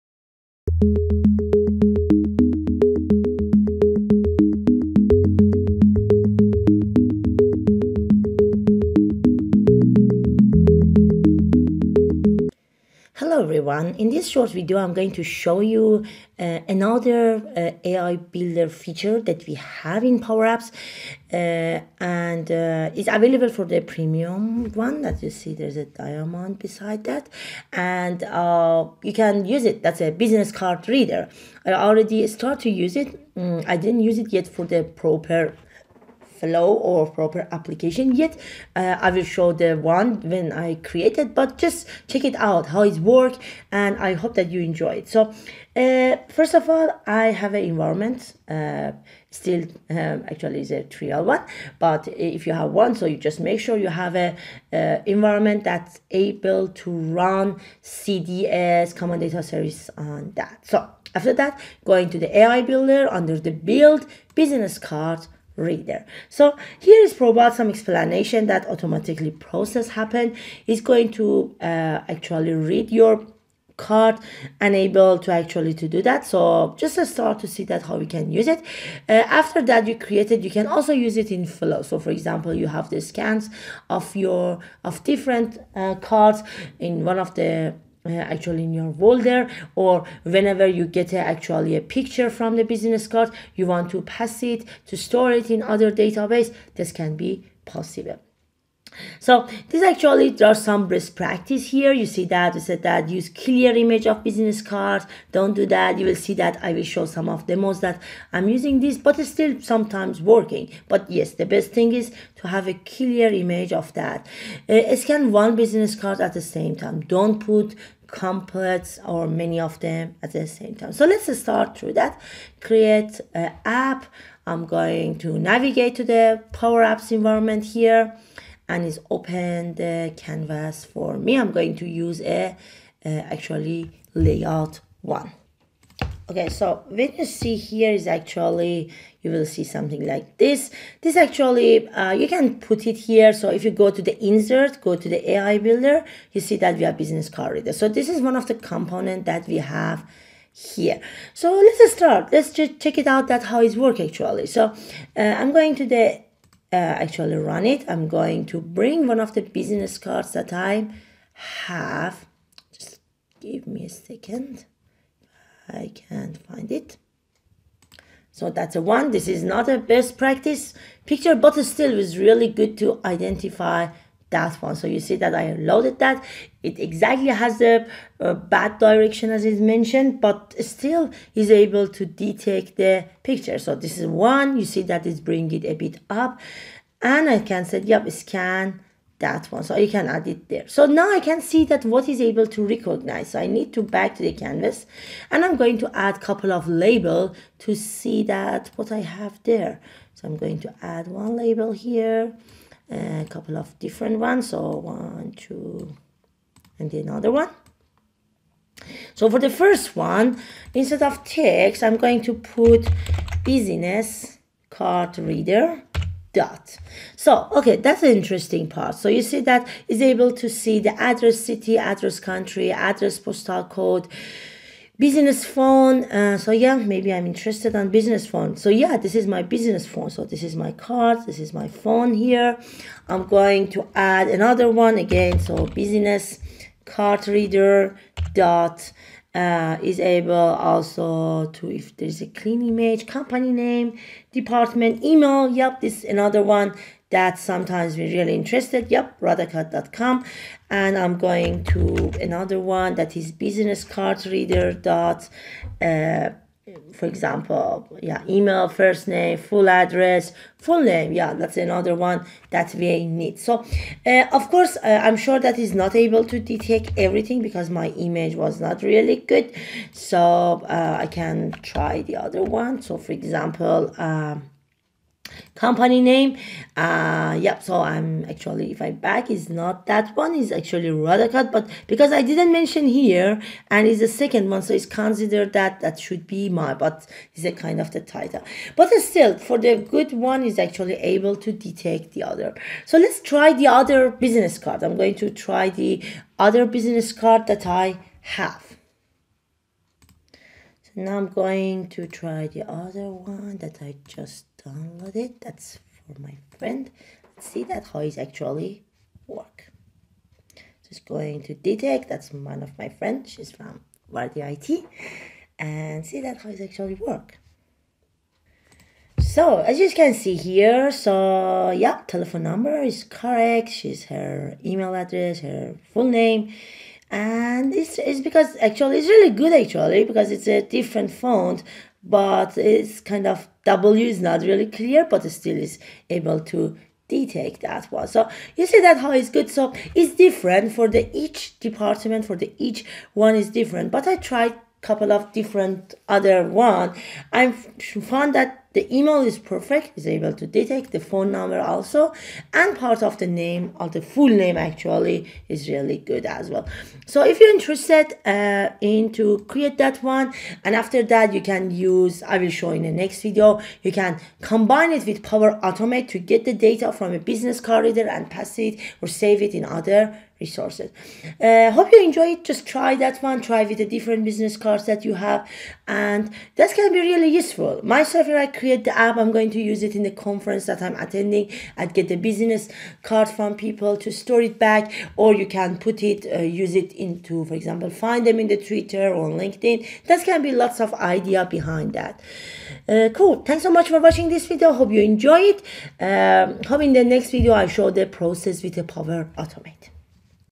Business Card Reader with Power Apps and AI Builder
in this short video, I have explained how to create a simple Business Card Reader with AI Builder